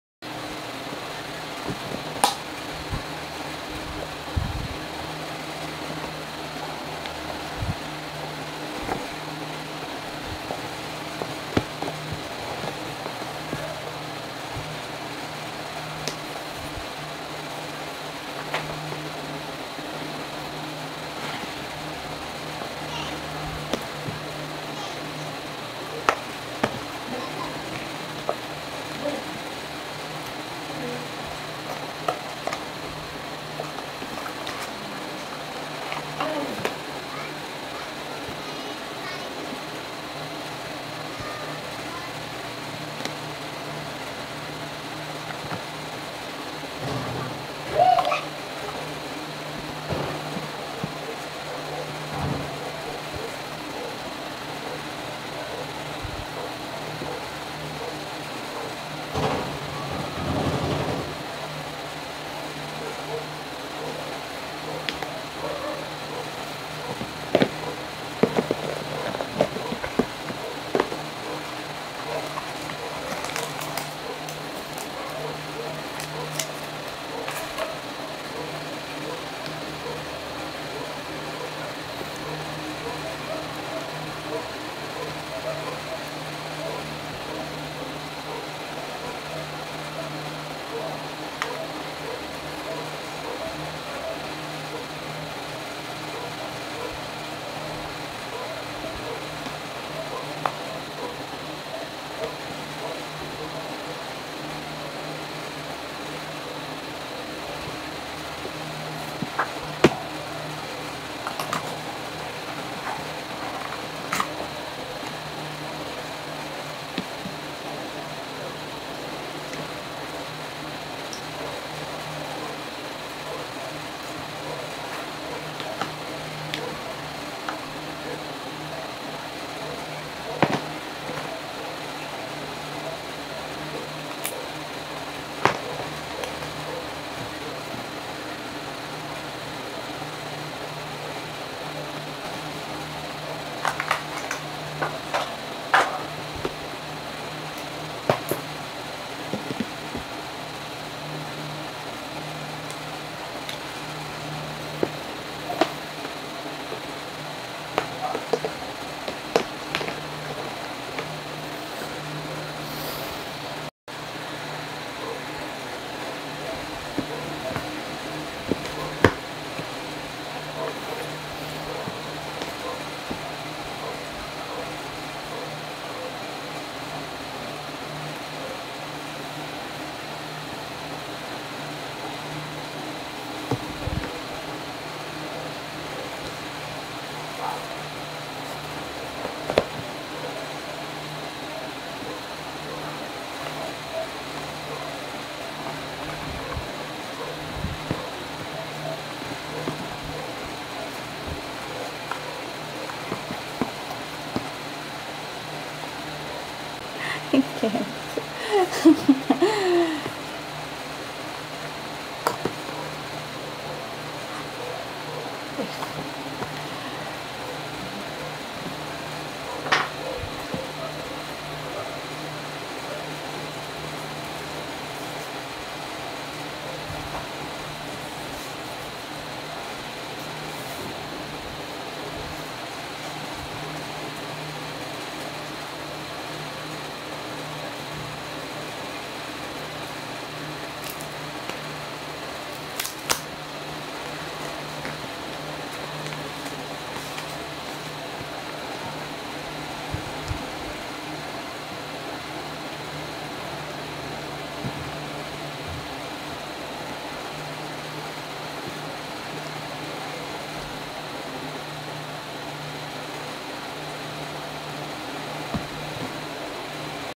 Life in details ✨️ Unboxing latest Samsung Galaxy A55 5G with freebies. It's cute but bulky!
First time doing "Unboxing content" I hope you like it. Let's see if I can use my new phone camera to take video, I hope it's good!

NOTED: If there might be any mispelled and  grammatical error pls let me know by leaving comments or message me.